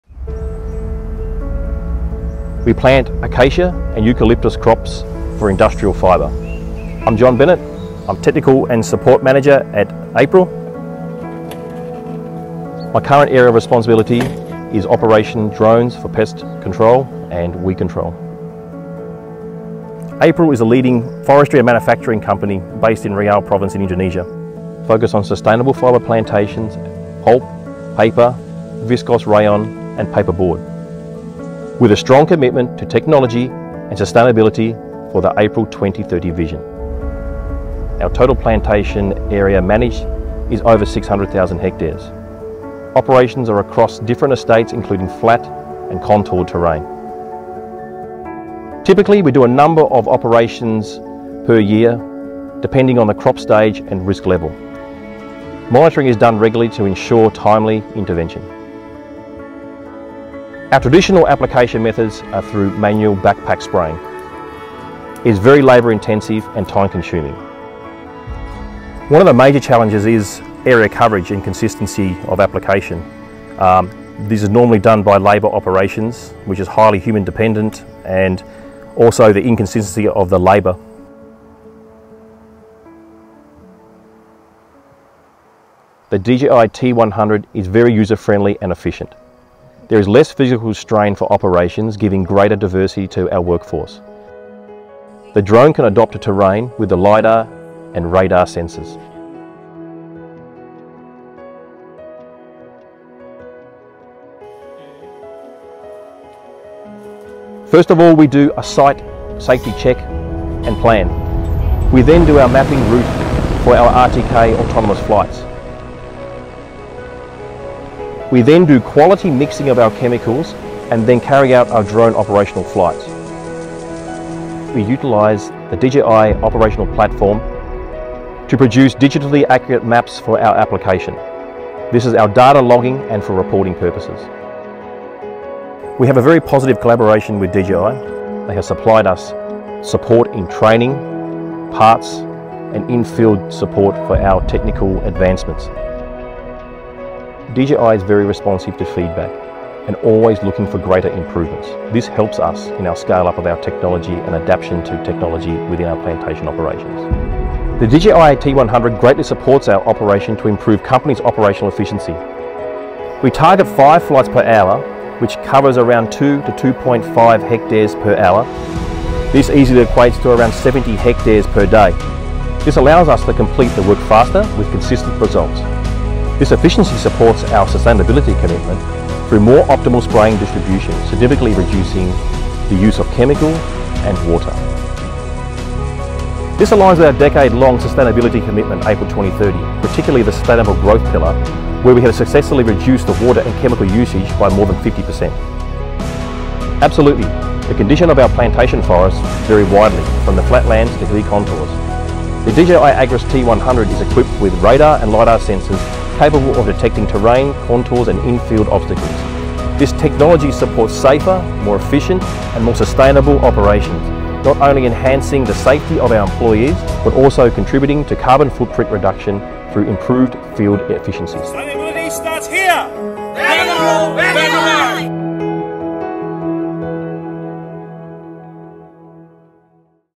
How DJI Agras T100 Powers Greener, Faster, More Efficient Forestry
How do you make a huge operation more green? 💚 Managing 600,000 hectares of forest used to be back-breaking work. Then the DJI Agras T100 showed up! 🚀 This drone is completely transforming how APRIL Group operates, making their massive forestry tasks faster, safer, and way more sustainable. 🌏
Come see what next-level efficiency looks like!